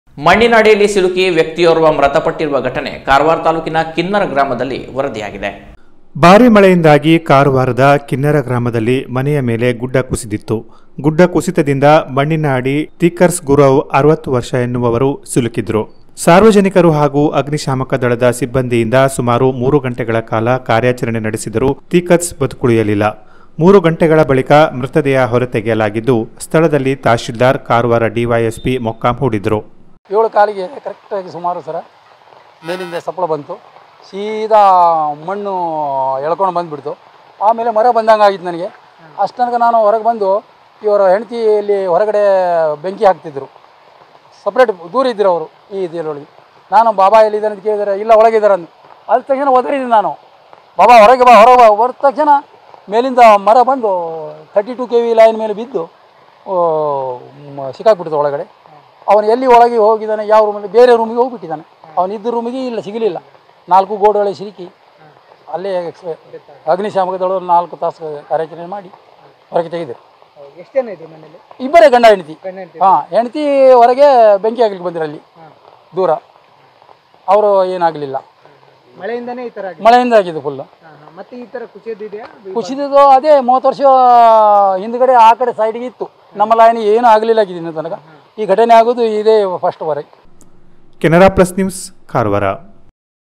ಮಣ್ಣಿನ ಅಡಿಯಲ್ಲಿ ಸಿಲುಕಿ ವ್ಯಕ್ತಿಯೋರ್ವ ಮೃತಪಟ್ಟಿರುವ ಘಟನೆ
16-7-24 ಮಣ್ಣಿನ ಅಡಿಯಲ್ಲಿ ಸಿಲುಕಿ ವ್ಯಕ್ತಿಯೋರ್ವ ಮೃತಪಟ್ಟಿರುವ ಘಟನೆ ಕಾರವಾರ ತಾಲೂಕಿನ ಕಿನ್ನರ ಗ್ರಾಮದಲ್ಲಿ ನಡೆದಿದೆ.